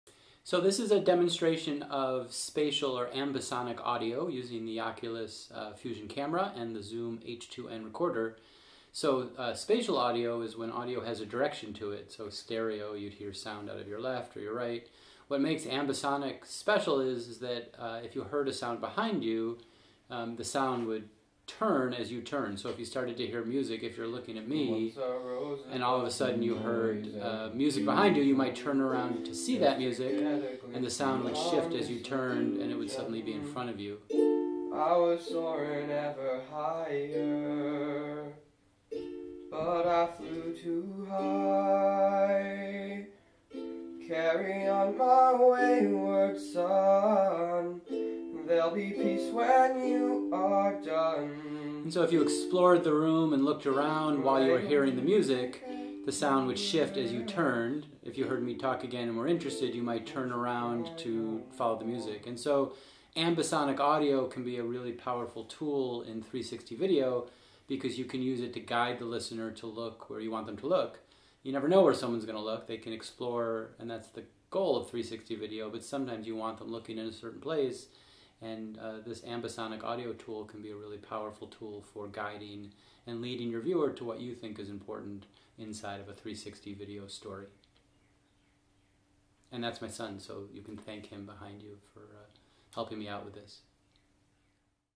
Ambisonic Demo h.265
Footage shot on a GoPro Fusion camera. Audio recorded on a Zoom H2N recorder. Edited in Adobe Premiere 2018 and exported as ambisonic VR footage using h.265